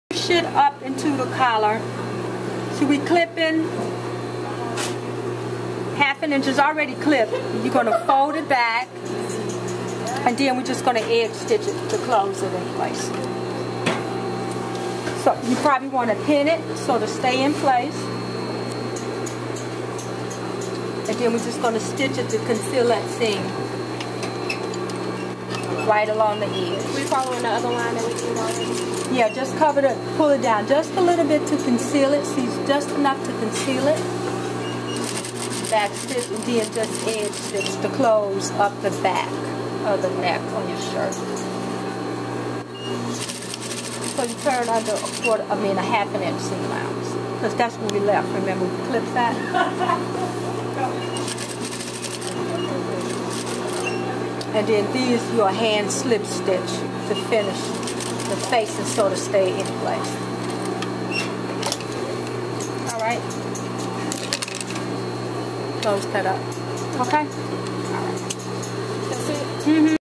Finsh Convertable Collar pt.2
Clip up to one half inch at side neck, turn under a half an inch, pin in place and edge stitch